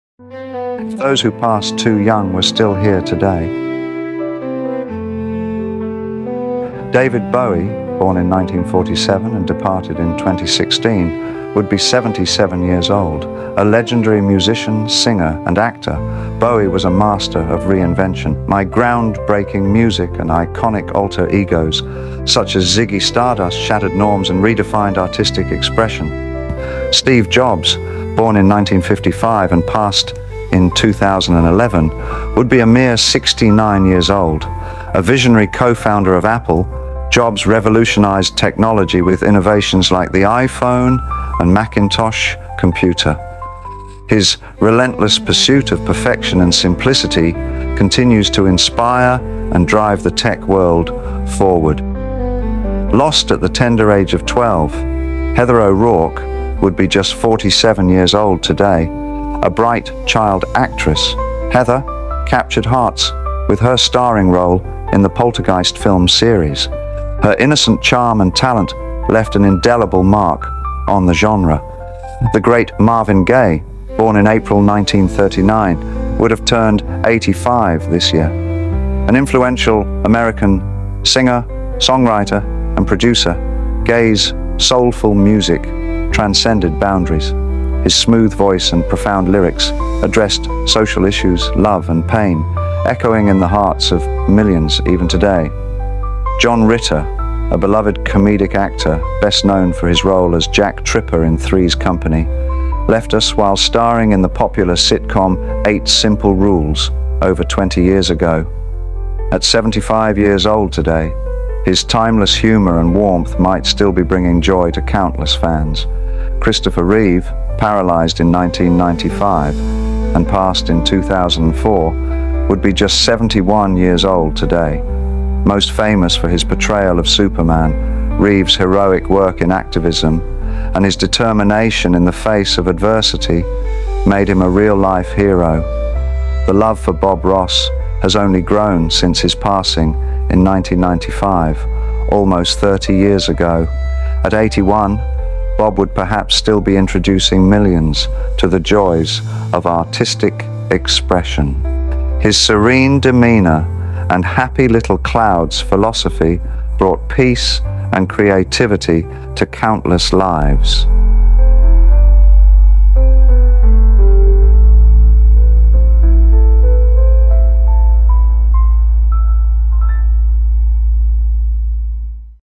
What Would They Look Like Today? Tributes to Those Who Passed Too Soon
Imagine a world where iconic figures like David Bowie, Steve Jobs, Heather O’Rourke, Marvin Gaye, John Ritter, Christopher Reeve, and Bob Ross were still with us today.

In this heartfelt tribute, we reflect on the incredible contributions and lasting legacies of these remarkable individuals. From groundbreaking music and revolutionary technology to unforgettable performances and inspirational activism, their impact continues to shape our world. Join us as we explore what their lives might look like now, honoring their memories and celebrating the indelible marks they left on history.

This is a continuation of a series that was popular on my channel on another platform, but with advances in AI technology, I can produce much more realistic portrayals to honor these individuals and all new episodes are on YouTube first.